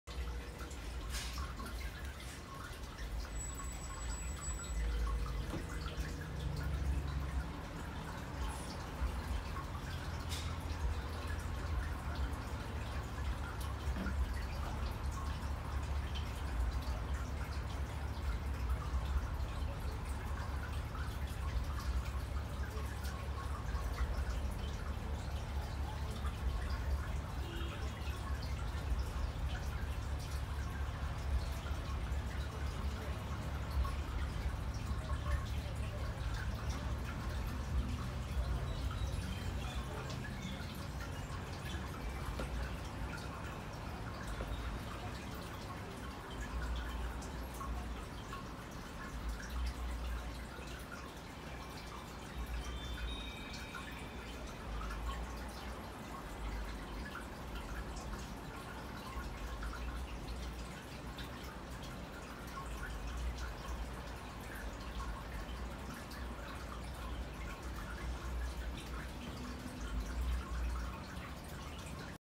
Over one year from set up of My 20 gallon Planted Aquascape Aquarium.
Cheery barbs have been busy breeding.  Cherry shrimp are doing well.  I have cut down my CO2 and see no difference in plant growth.